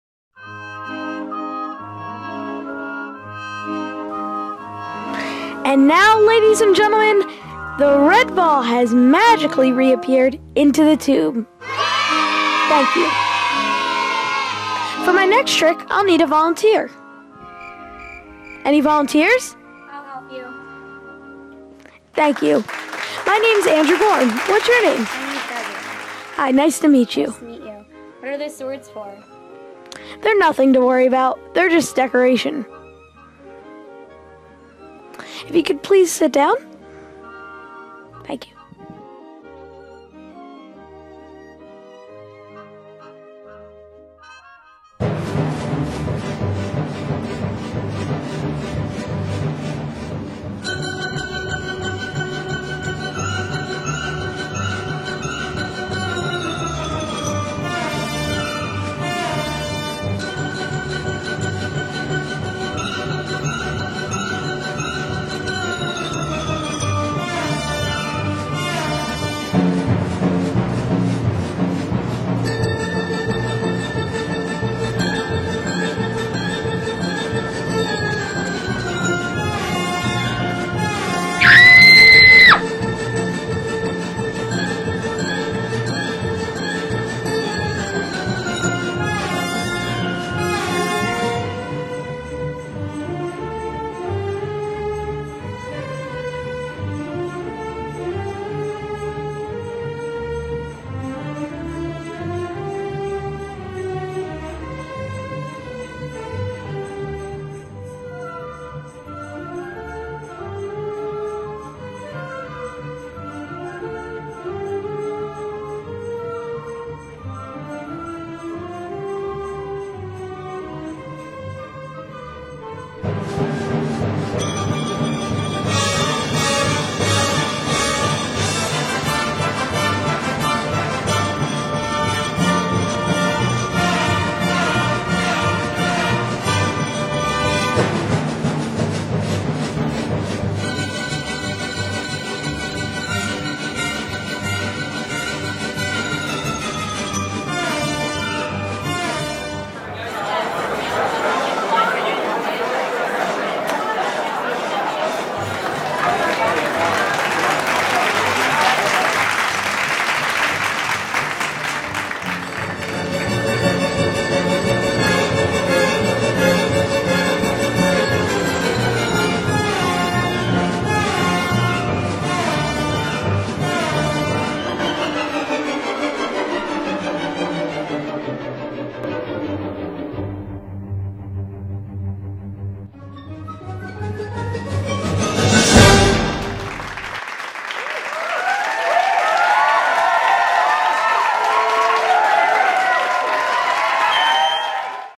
Rick Saphire's Magic Shop - Dagger Lady
The Amazing Andrew plunges nine metal daggers through the head of his beautiful female assistant. Where did her head go? The Dagger Chest Illusion is available at Rick Saphire's Magic Shop.